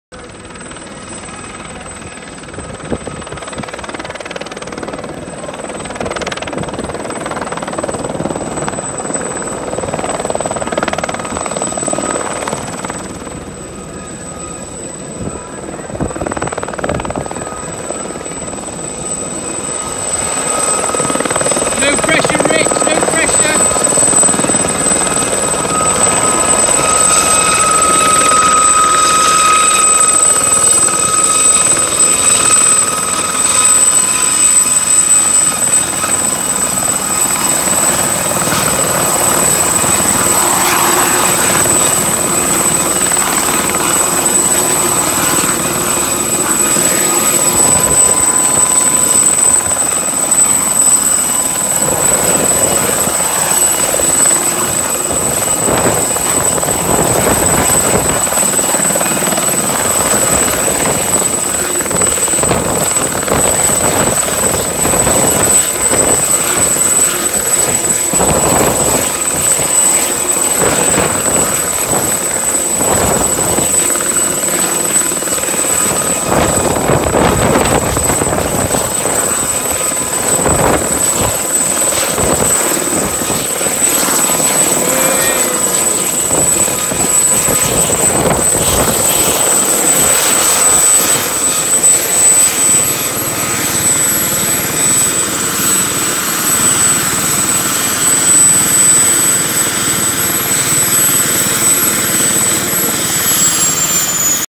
5 AAC Gazelle arrives at RIAT 2018
The British Army Gazelle helicopter arrives perfectly on time at RAF Fairford to take it’s place on the Static display stands.